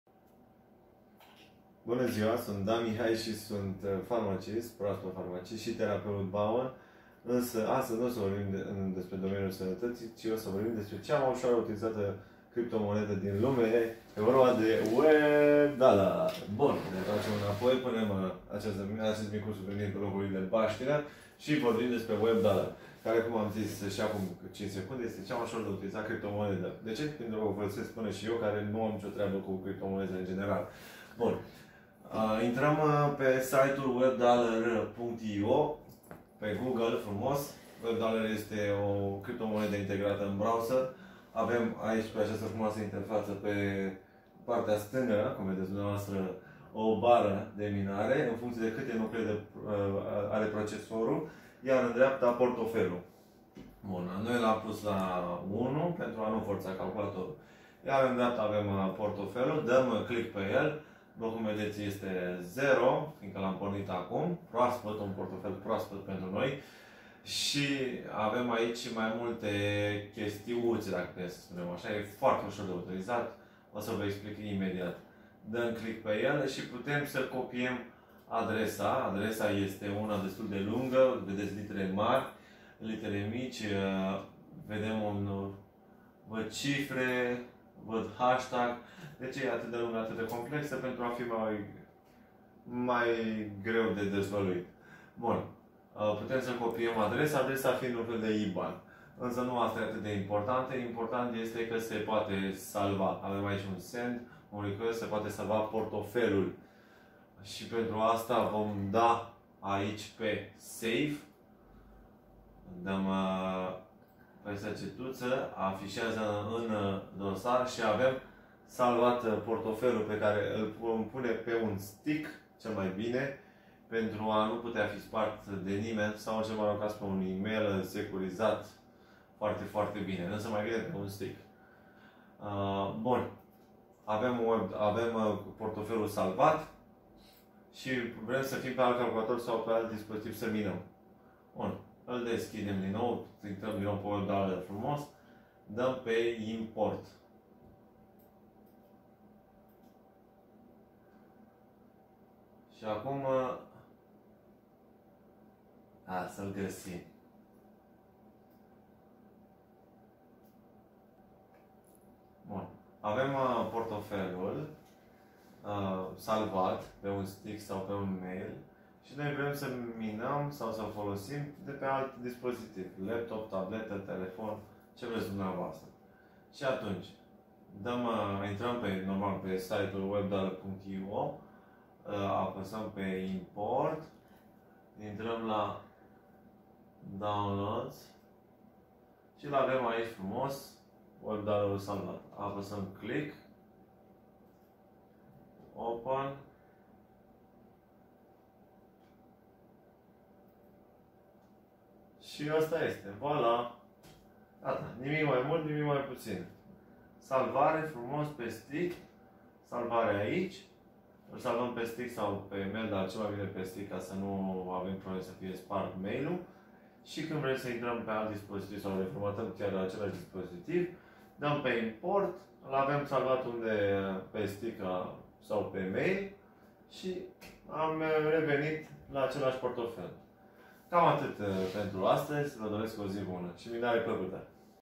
Cum salvezi portofelul criptomonedei WebDollar?
WEBD$gDKZ7LXcn5NsY7M2pypSquQnBEqb8h3Q#f$

Pentru a salva adresa de portofel pentru criptomoneda WebDollar ai nevoie de 3 click-uri:
2. deschide portofelul din dreapta jos;
3. apasa descarca;
Dupa salvare pe calculator se recomanda:
4. copiaza fisierul într-un loc sigur (stick, email securizat);
5. pentru utilizare, se poate importa pe orice dispozitiv (calculator, laptop, tableta, telefon).